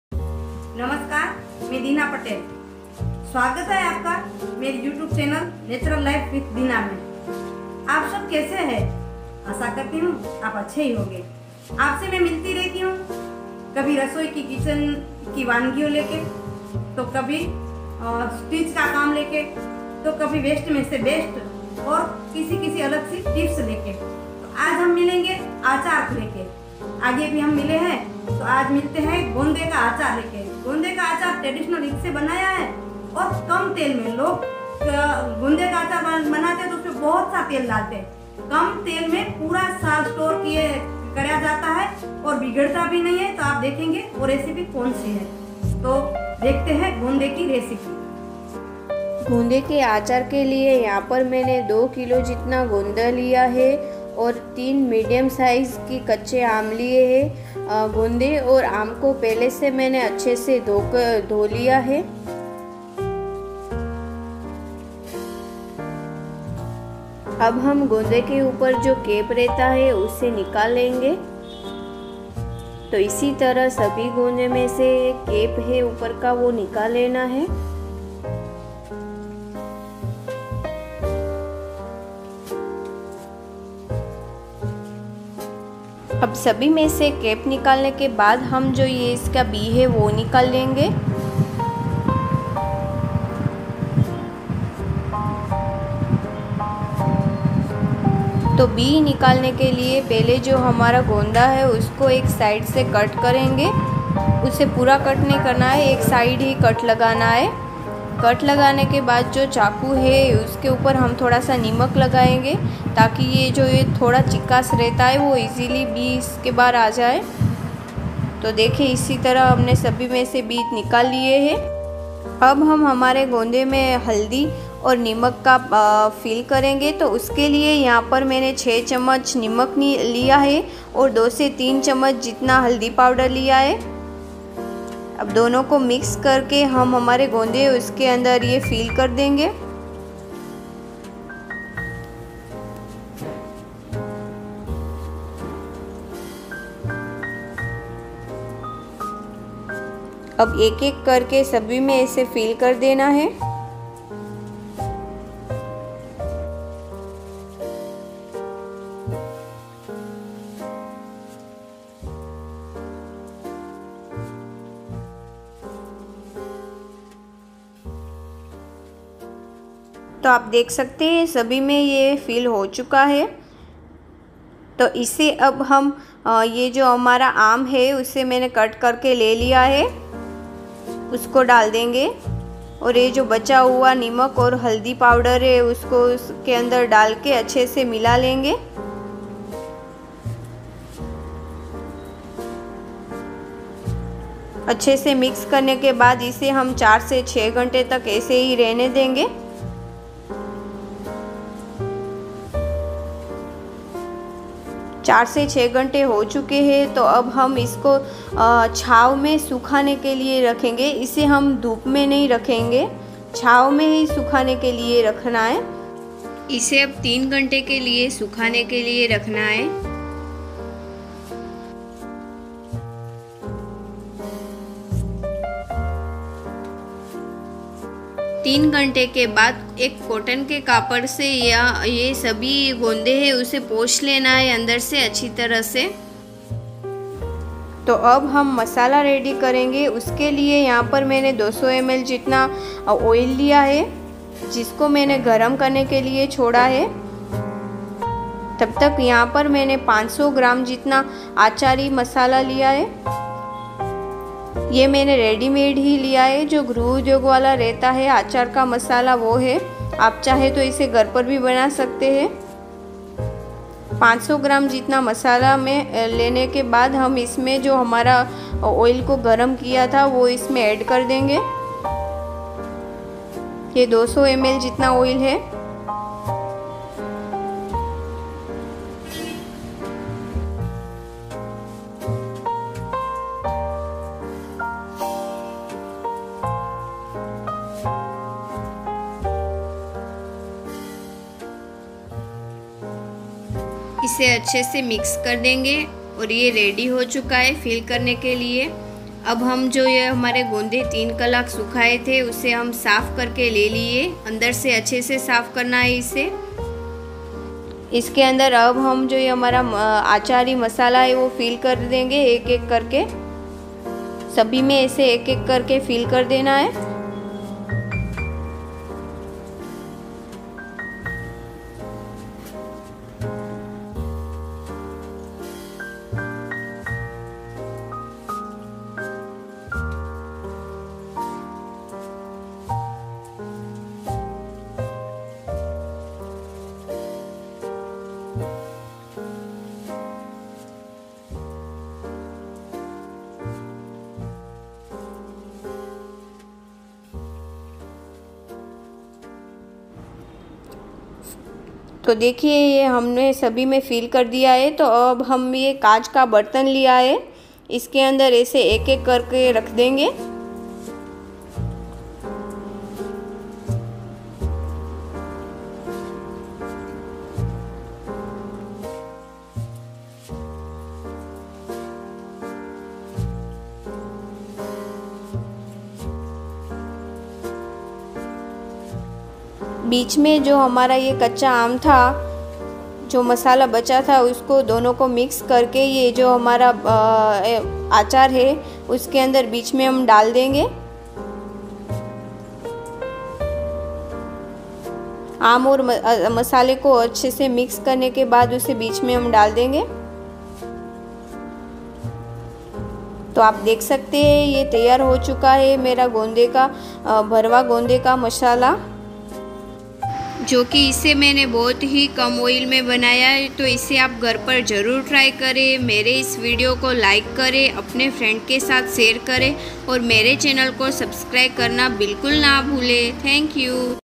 कम तेल के बनाए गुंदे का आचार | लसुदे का अचार | gunde ka aachar recipe| natural life with dina
गुंदे के आचार को कम तेल मे कैसे बनाए मैंने आज मेरे वीडियो में शेयर किया है।  जिसे बनाना बहुत ही आसन है और आप ऐसे आराम से 1 साल तक स्टोर कर सकते हैं।तो वीडियो को जरुर देखे।

ingredients:

गुंदे: 2 kg
नमक: 6 tbsp
हल्दी पाउडर: 2 tbsp
तेल: 200 ml
अचार मसाला: 500gm